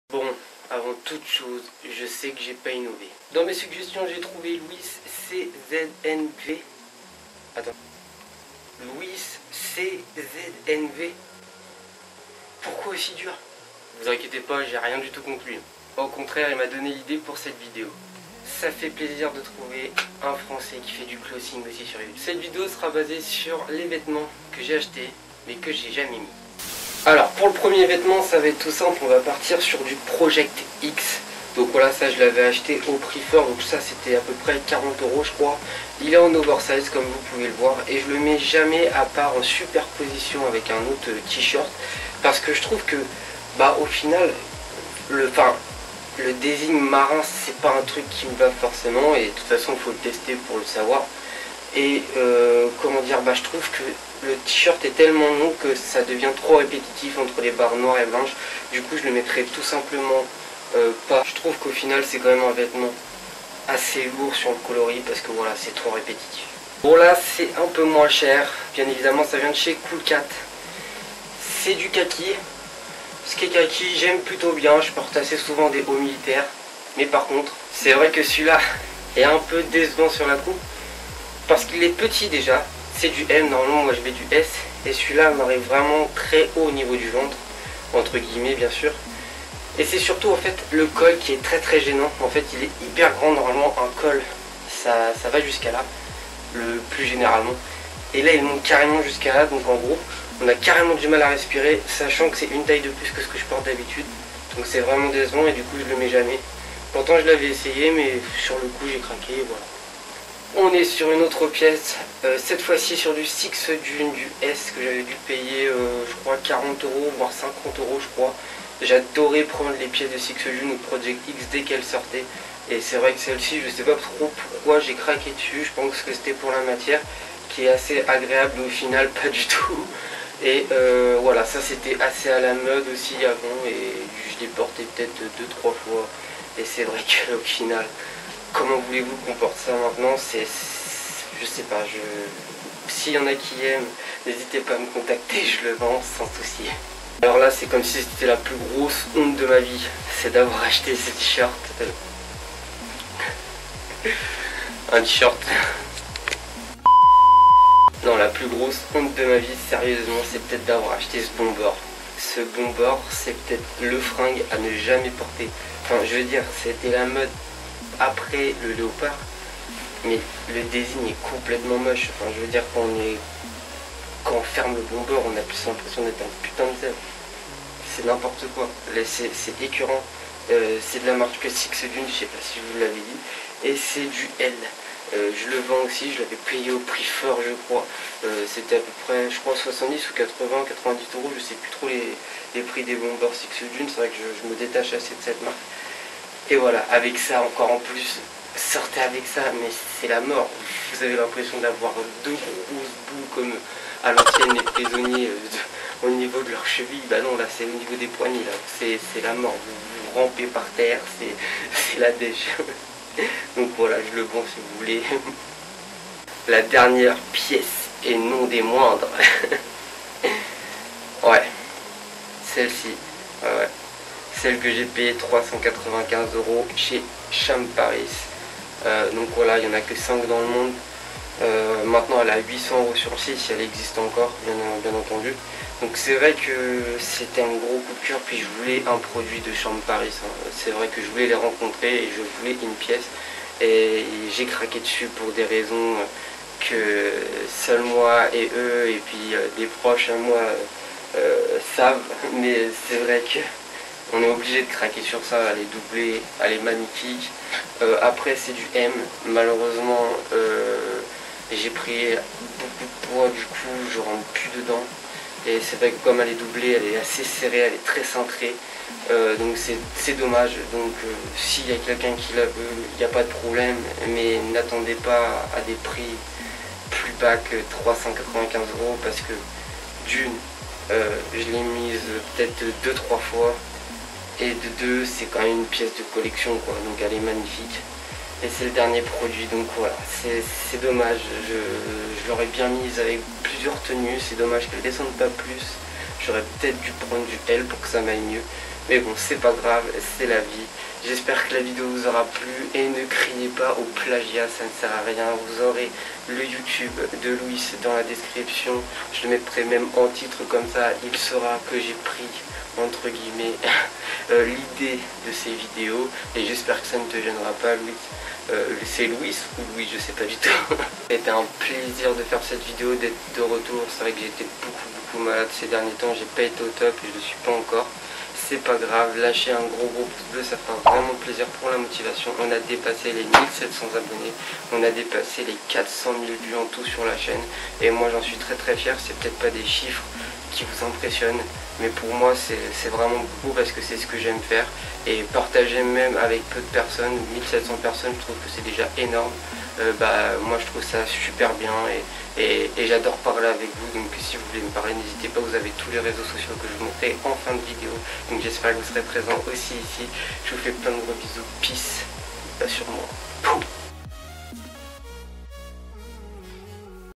LES VÊTEMENTS QUE J'AI ACHETÉ MAIS JAMAIS MIS l LOUIS CZNV
Petite vidéo qui fais du bien , qui change. Voici donc les produits que j'ai acheté mais que je ne met jamais!